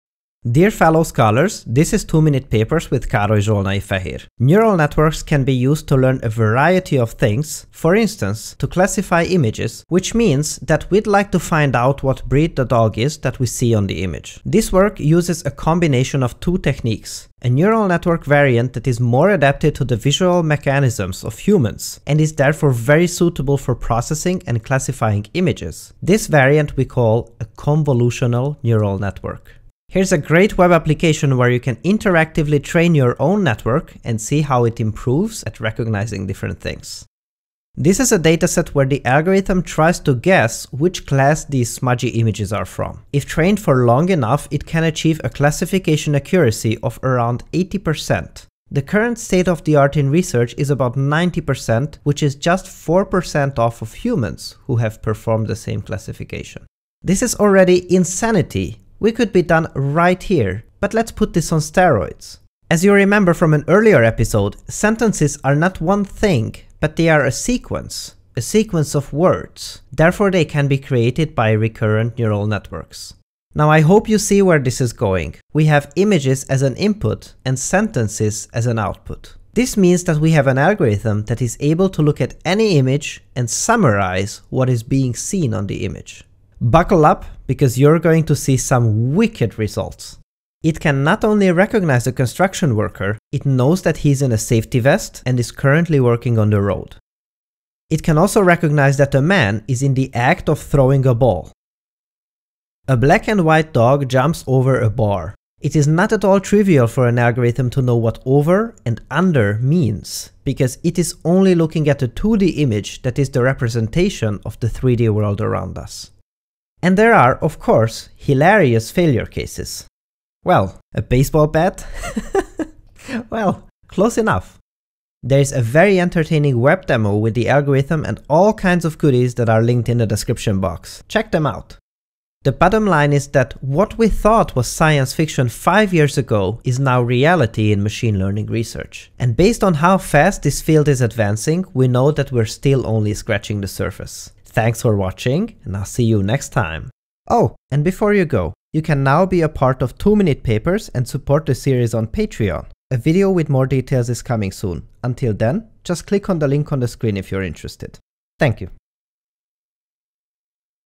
Recurrent Neural Network Writes Sentences About Images | Two Minute Papers #23
This technique is a combination of two powerful machine learning algorithms:
- convolutional neural networks are excellent at image classification, i.e., finding out what is seen on an input image,
- recurrent neural networks that are capable of processing a sequence of inputs and outputs, therefore it can create sentences of what is seen on the image.

Combining these two techniques makes it possible for a computer to describe in a sentence what is seen on an input image.